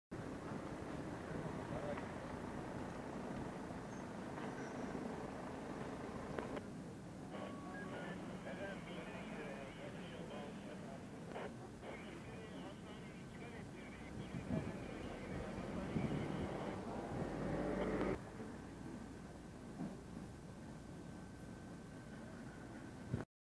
Bafra`da merdivenden düşen şahıs yaralandı
Samsun`un Bafra ilçesinde merdivenden düşen şahıs yaralandı.